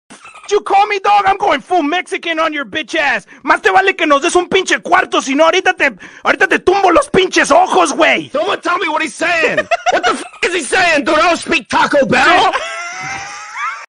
P03 bout to get full Mexican (Inscryption Act 2 Slight Spoilers)
TRIED THE 30 MINUTE ANIMATIC  CHALLENGE, (forced by a friend o mine) kinda proud! :D

Also, first time drawing Magnificus and Leshy (upper body), I think I did pretty okay :)

Original: ??? I think it's on TikTok so if anyone could give the original I'd be thankful :D

EDIT: OK OK I GET IT IT'S NOT ON TIKTOK PLEASE STOP LOL